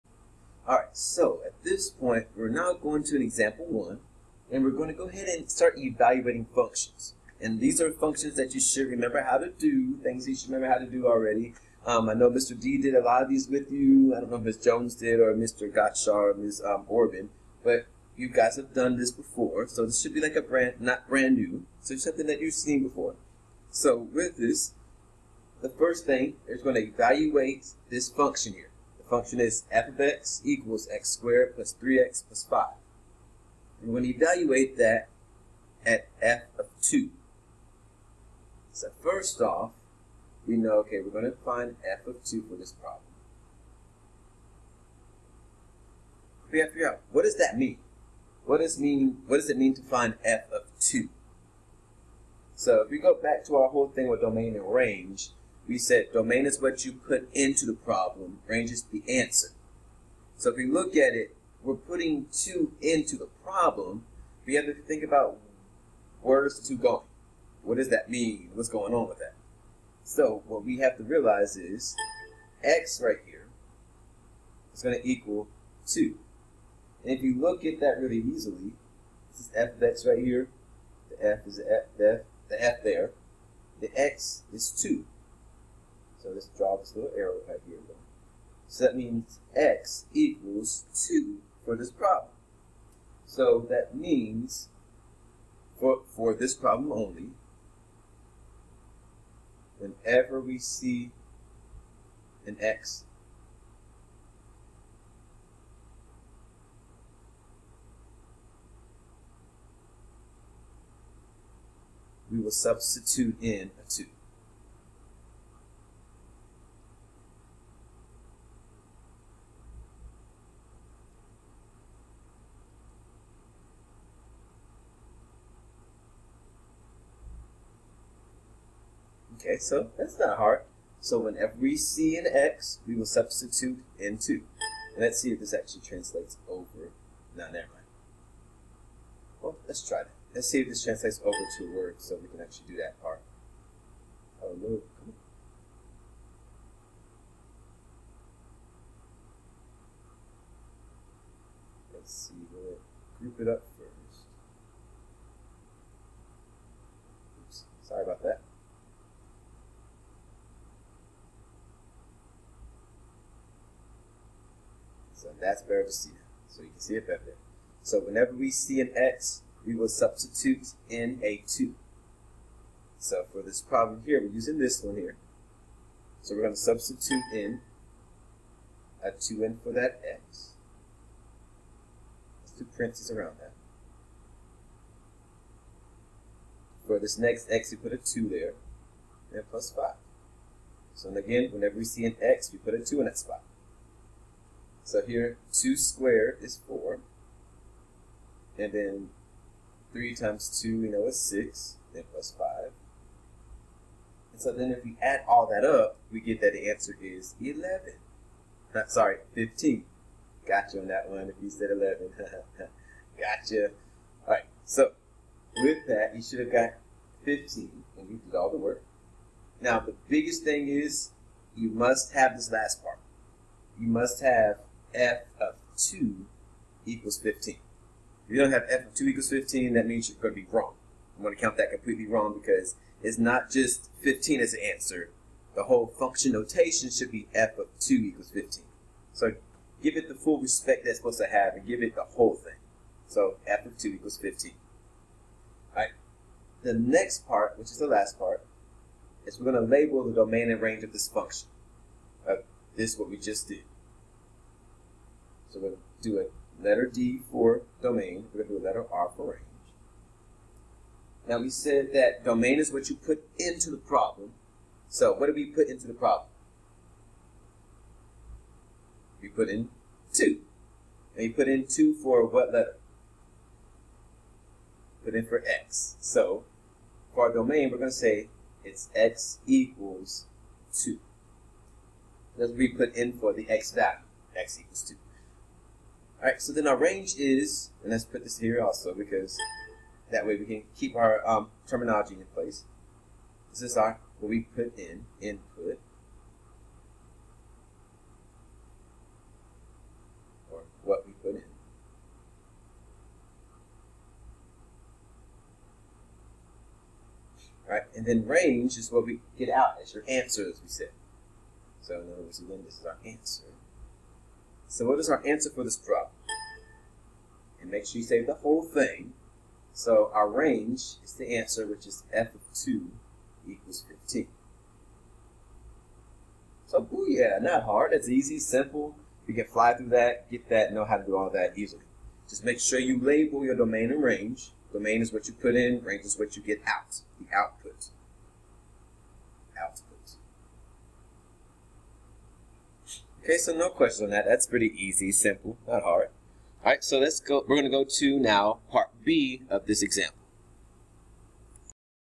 Example 1 a
Simple evaluating functions with a given x-value